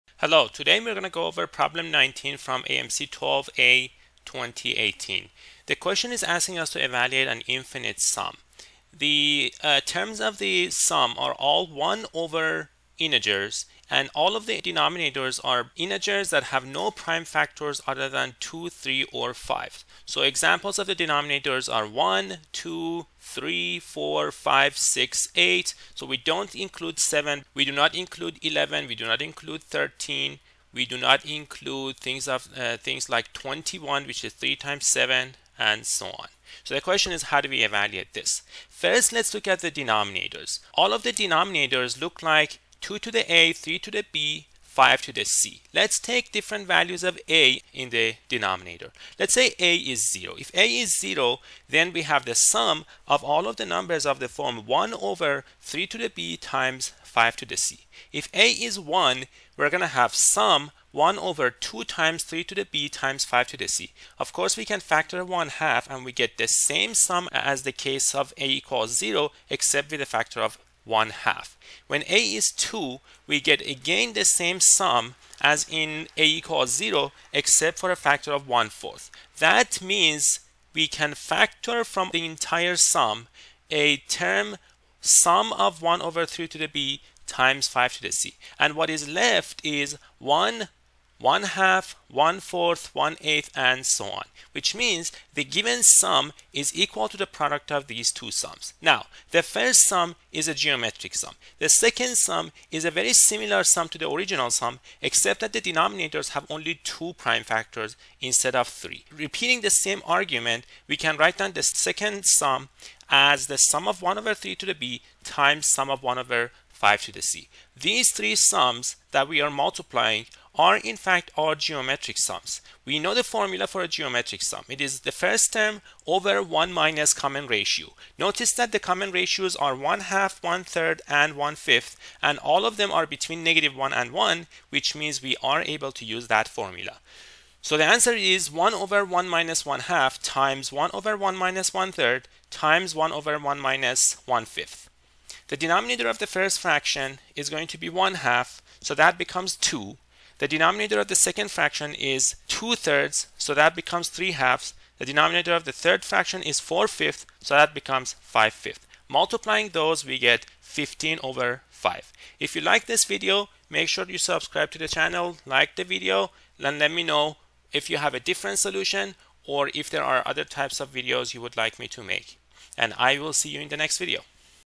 AMC 12A, 2018, Problem 19; Breaking down a Math Competition Sum!
I solve problem 19 from the American Math Competition 2018 (AMC 12A, 2018, Problem 19). The main tool is the geometric sum formula.